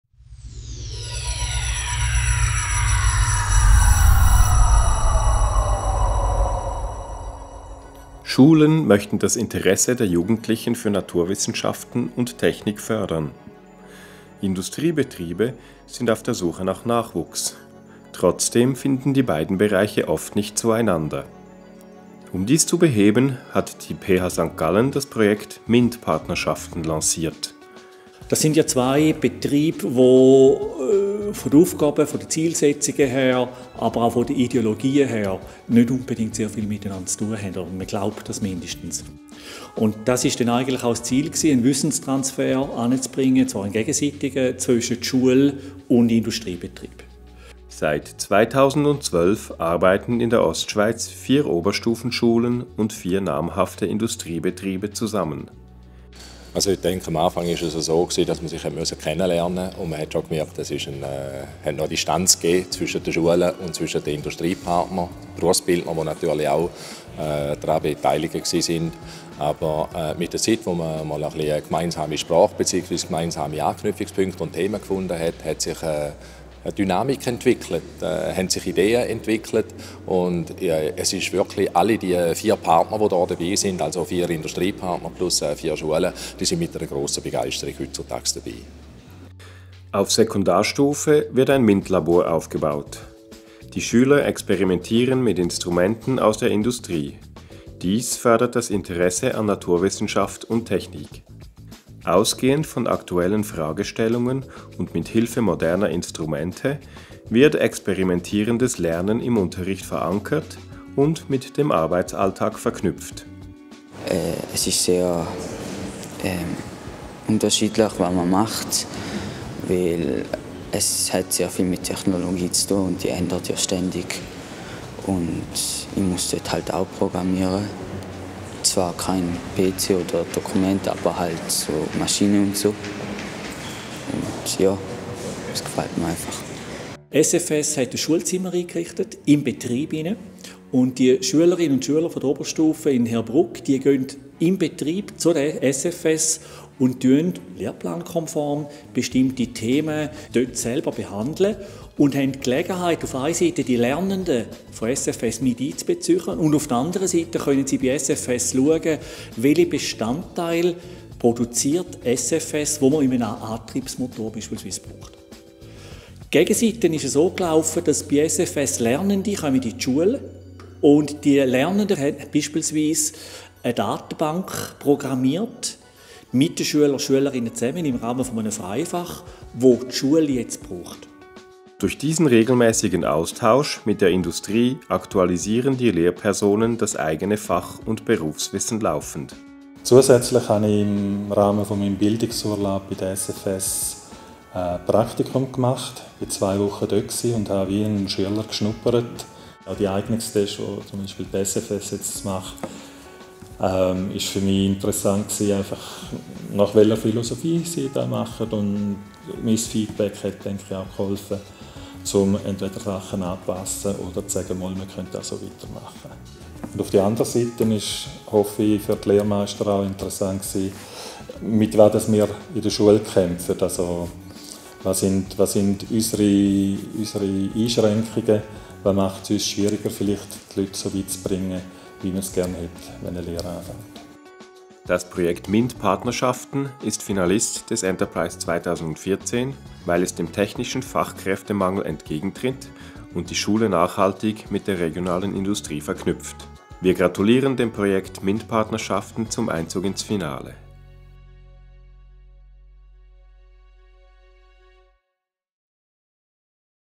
Gewinn ENTERPRIZE 2014
SFS die OMR, je drei Industriepartner (Hilti, Geberit und Bühler) und Oberstufenschule gewinnen zusammen mit der PHSG mit dem gemeinsamen MINT-Projekt den Enterprize 2014.